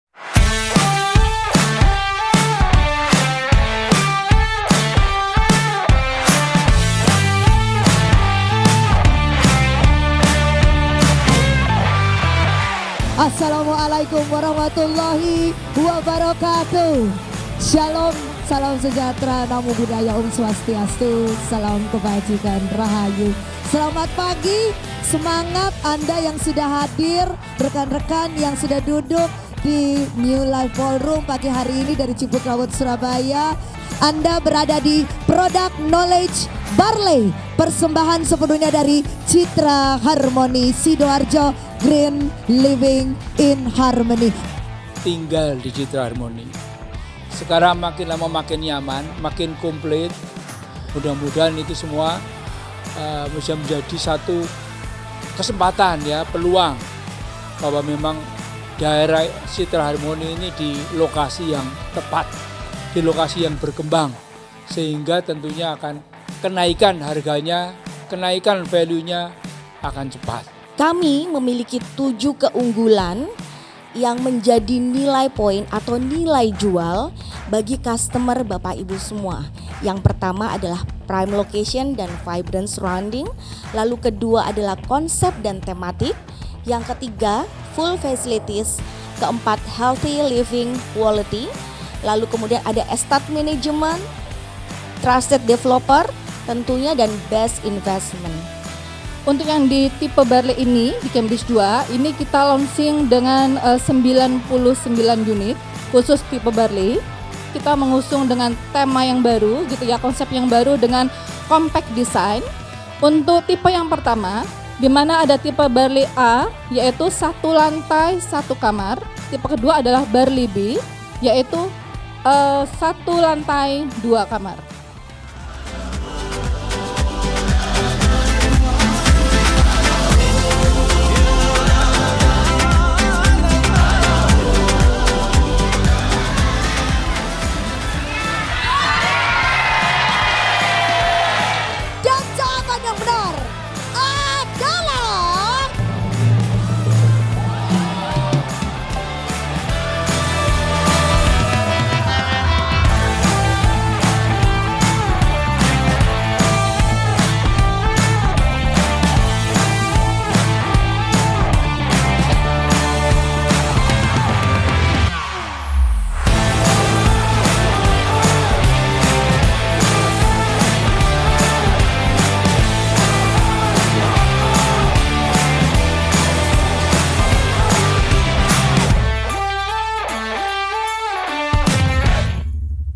PRODUCT KNOWLEDGE CAMBRIDGE 2 | Rumah Type BARLEY
CitraHarmoni Sidoarjo mengeluarkan rumah type terbaru dengan Cluster Cambridge 2

Booking sekarang!! Dapatkan Promo Special🥰
✅️ Harga 600 Juta-an
✅️ DP Mulai 10 Juta
✅️ Angsuran KPR mulai 3 Juta
✅️ Subsidi Asuransi Jiwa*
✅️ Bebas Semua Biaya*